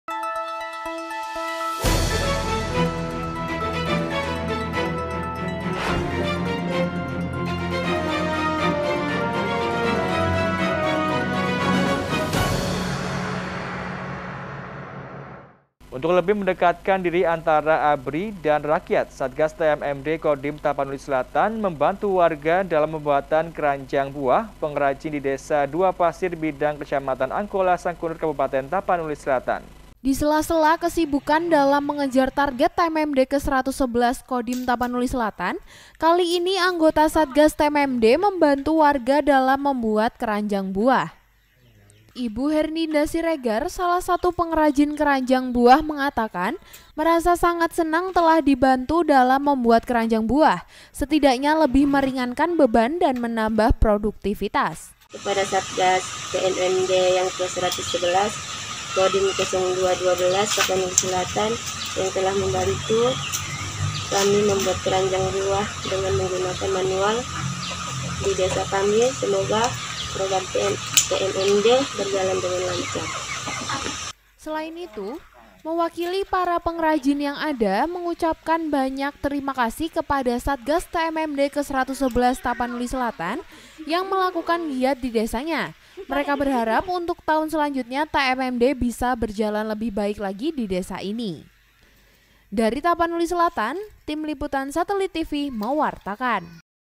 SATGAS TMMD MEMBANTU WARGA BIKIN KERANJANG BUAH
UNTUK LEBIH MENDEKATKAN DIRI ANTARA ABRI DAN RAKYAT
SATGAS TMMD KE 111 KODIM 0212 TAPANULI SELATAN  MEMBANTU WARGA DALAM PEMBUATAN KERANJANG BUAH / PENGRAJIN DI DESA DUA PASIR BIDANG KECAMATAN ANGKOLA SANGKUNUR KABUPATEN TAPANULI SELATAN MENYATAKAN SANGAT SENANG DI BANTU ANGGOTA SATGAS.